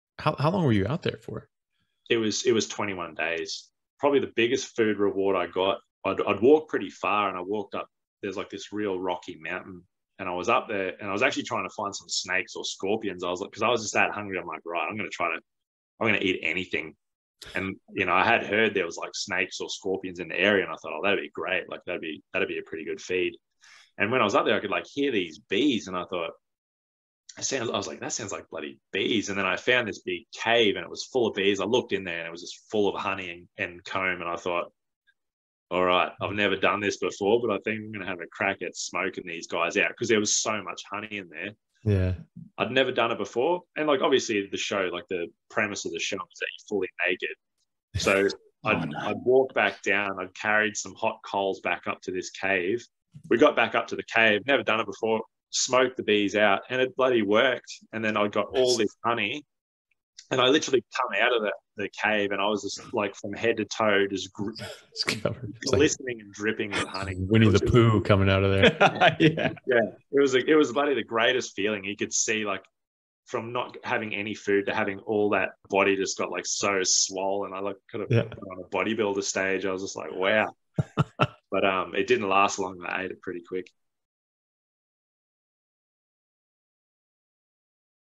21 days in the wild on the show Naked and Afraid
21 days fending for himself in the wild on Naked And Afraid. This is my only exception to eating off carnivore with honey etc, when you're out in the wild and need to survive.

How To Carnivore Episode 30 with real bushman Adam Kavanagh!

Adam Kavanaugh gave up a high paying career in the mining industry of Australia to live out in nature as our ancestors have since time immemorial.

Adam faced a number of health issues and turned to a "caveman diet" and lifestyle, which completely reversed them. After travelling Australia twice and spending 4 months in the wilds of Africa, he decided to make this a lifestyle and left his career in the mines for the Outback.  There he hunts and fishes with the Aboriginal Australians, takes adventure hungry people out on survival tours, and has even made it through Naked and Afraid.  Come hear his amazing story and get a glimpse of what life is like when you go back to nature.  Enjoy!

Find Adam:
Adam can be found under his name Adam Kavanaugh on multiple platforms, as well as through the following links